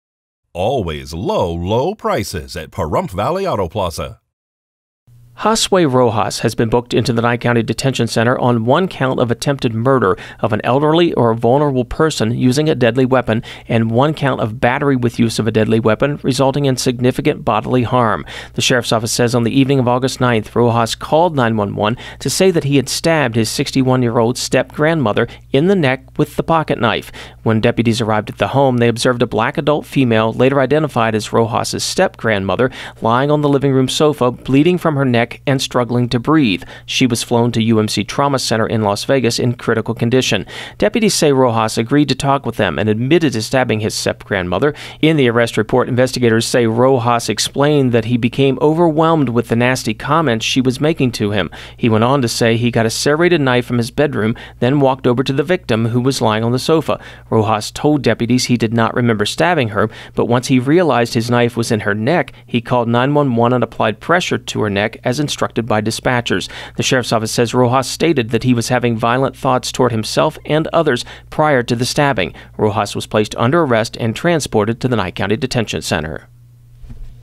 08-25-2021 Pahrump: Josue Rojas Under Arrest For Stabbing on Leslie Street
The Nye County Sheriff's Office has released more information about a stabbing earlier this month on North Leslie Street in Pahrump, including the identity of the suspect and the charges he's now facing.